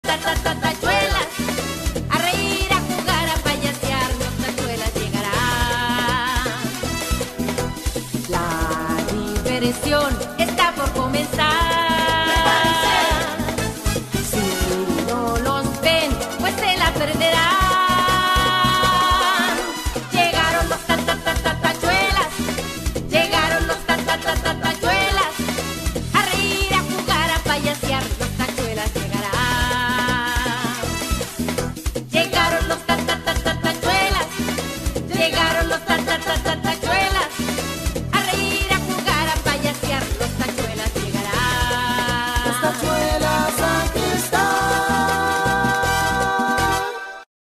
Circo Los Tachuelas - Viña Del Mar 2023
Drone desde ViñaDelMar enero y febrero en el EsteroMargaMarga